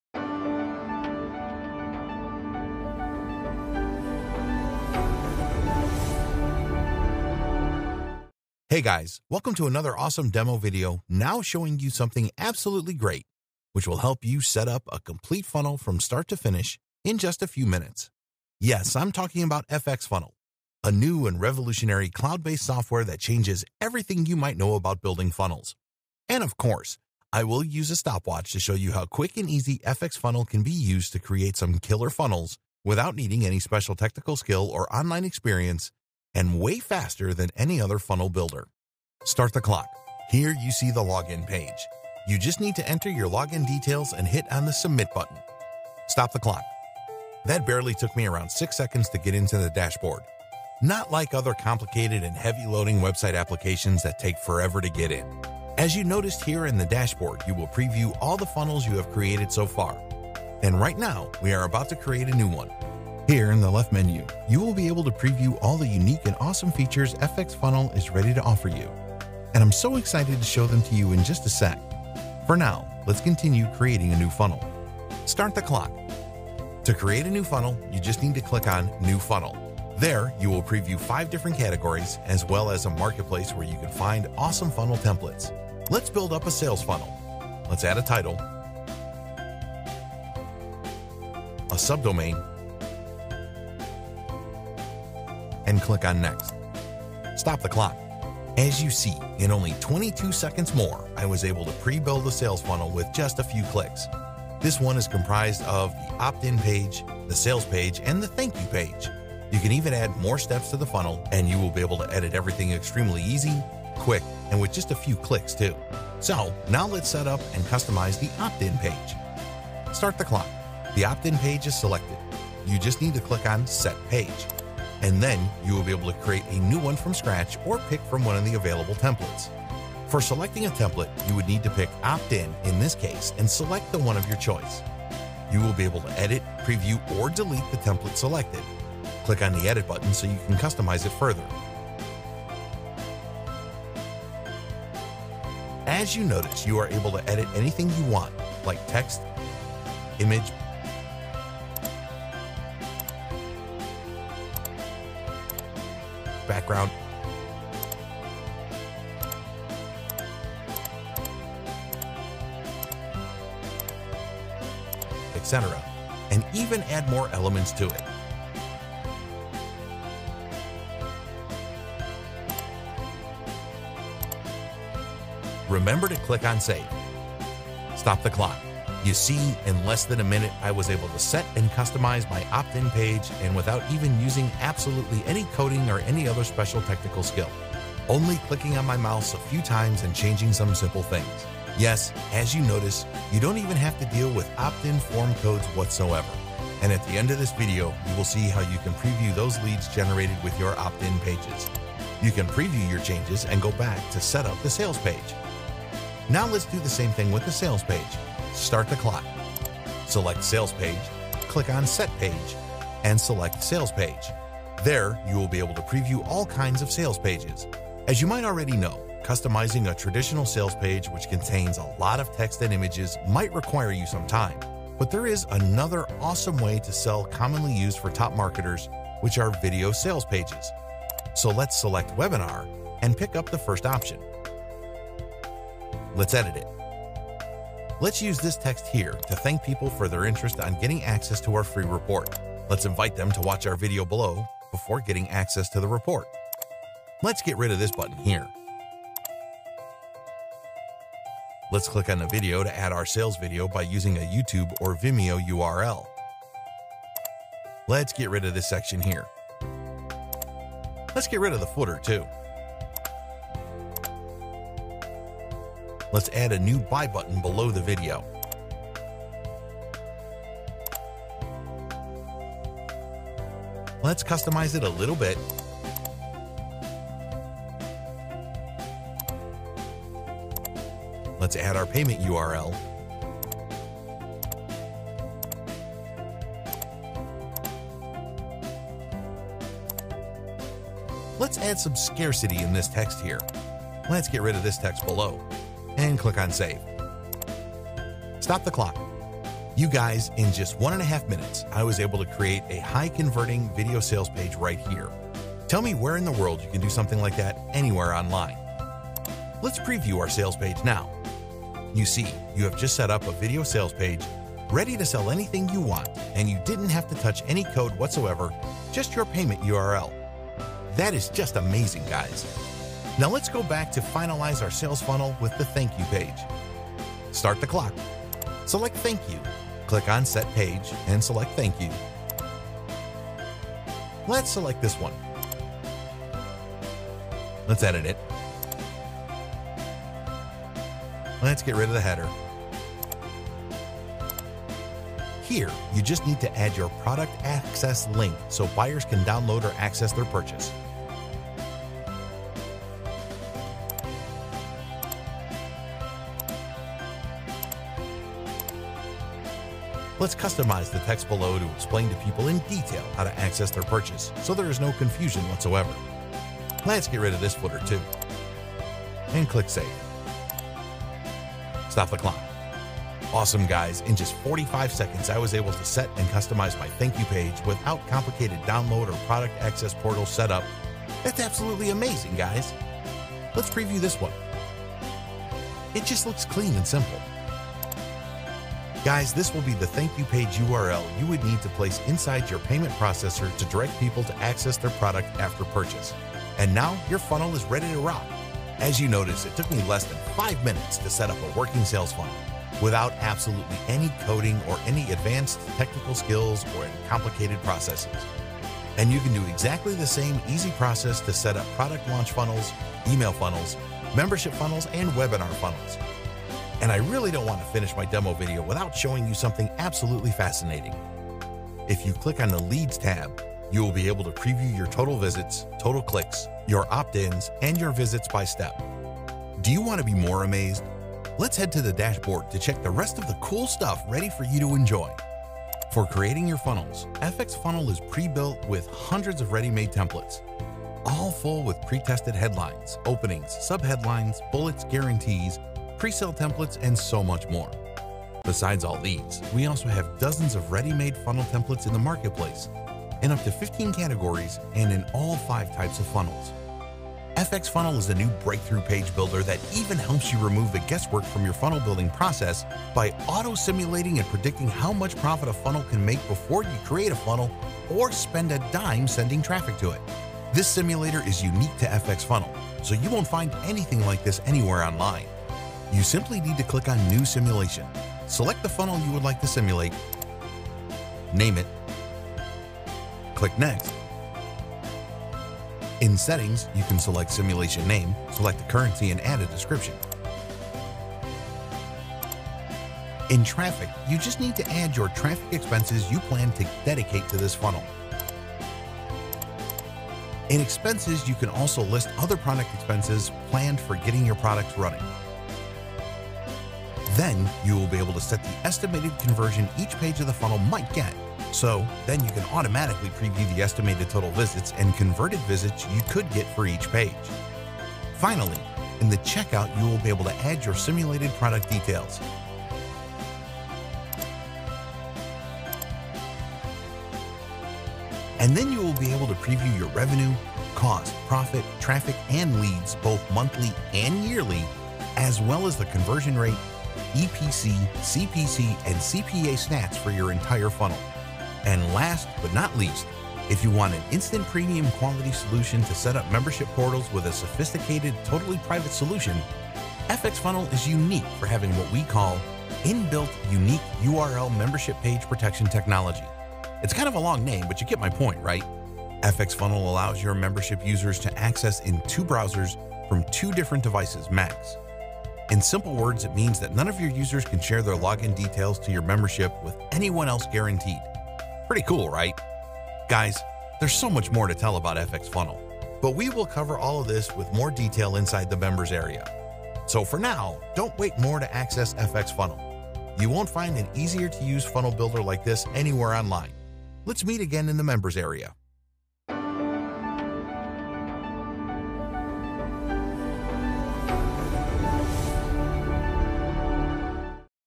FxFunnel Click Create Profit- Worlds 1st Smartest Funnel + Membership Builder
The First AND Only Smart Funnel Builder That ACCURATELY Predicts The FUTURE.

FXFunnel is a new breakthrough Funnel + Membership Page Builder that helps you remove the  guesswork from your funnel building process by auto simulating & predicting how much profit a funnel can make before you create a funnel or spend a dime sending traffic to it.

Plus with our inbuilt Unique URL membership page protection technology, none of your subscribers can share Login details to your membership page with anyone else. This is because the moment a user opens up the member area on their device it locks onto it and cannot be opened elsewhere unless they seek permission from you.

In Fact, FXFunnel is a revolutionary new cloud-based software that changes everything because it finally makes it easy for anyone, even a total newbie to create a membership site, sales funnel, and securely deliver products that someone has purchased…

Features
FXFunnel Is An All-In-One Platform That Gives You Almost Endless Possibilities…

Create a membership website and charge monthly, recurring fees
Setup a webinar funnel that makes mid and high-ticket sales in just minutes
Use the built-in tools to create a sales funnel that actually converts visitors into buyers
Sell physical or digital products with FXFunnel
Build stunning funnels without any design or technical skills needed
FXFunnels integrates with various payment processors and email marketing platforms
Securely hosted in the cloud so there’s never anything to install or update

Just 4 Steps To Creating Top-Notch Membership Funnels That Stuff Your Pockets With Cash

Step #1 – Login to the easy-to-use dashboard and create your product
Step #2 – Setup your membership portal (it’s point and click simple)
Step #3 – Auto-predict profit potential of your Fx Funnel before building your funnel using the built-in tools (you don’t need any technical skills or experience to create a stunning funnel that converts like crazy)
Step #4 – Publish your funnel and enjoy the profits!